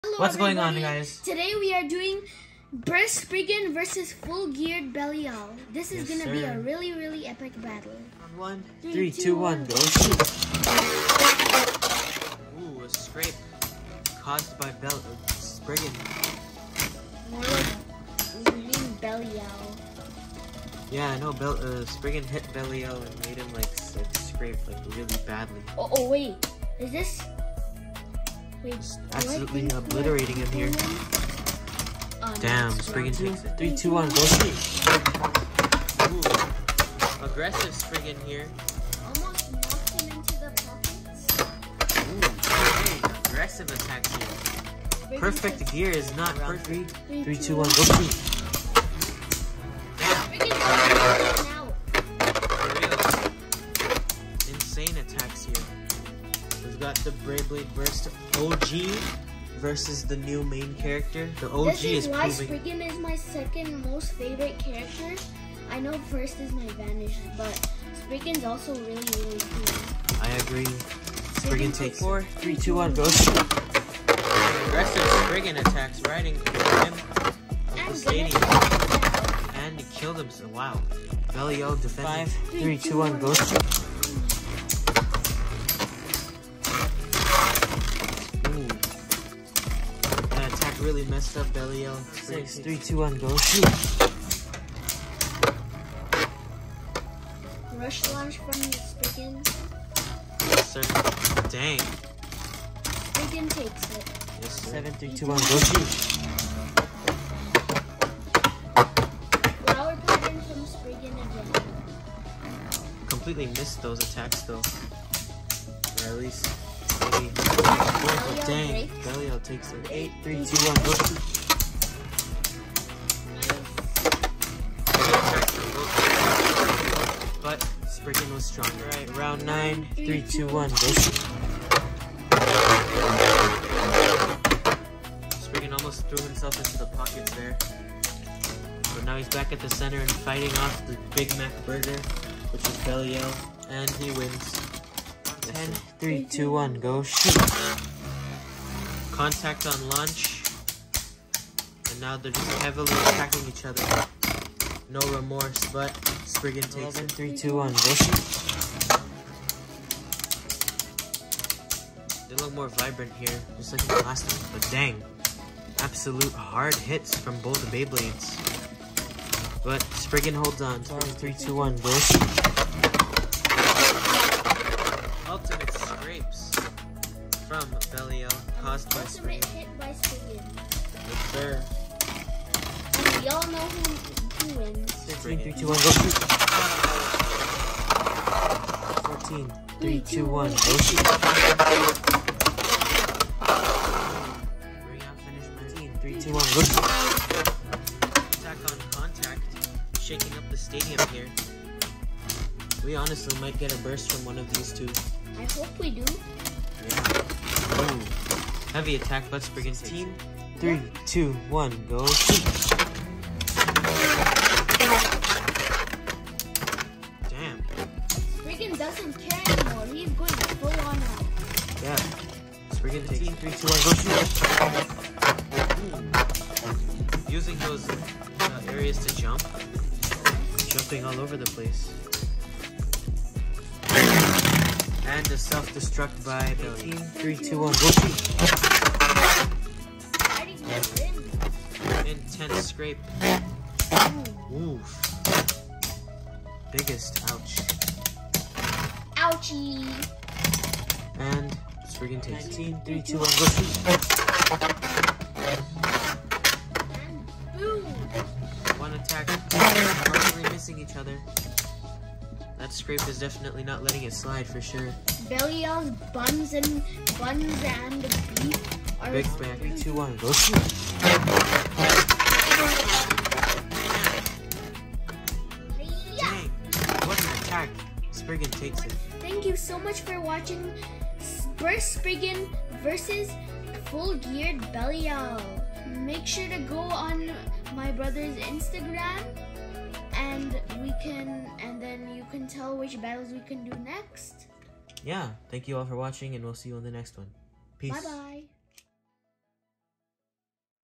Ultimate Gear Belial vs Burst Spriggan | Beyblade Burst BU/Quadstrike | Shu vs Bel (REMATCH)
In this video, Belial has a rematch against Spriggan after losing previously. But this time Belial has all evolution gears

Parts⬇️

Layer: Divine             Layer: Burst
BU Core: Belial3       BU Core: Spriggan
Armor: 3                    Armor: 8
Disc: Nexus              Disc: Spread’
Driver: Adventure    Driver: Quattro’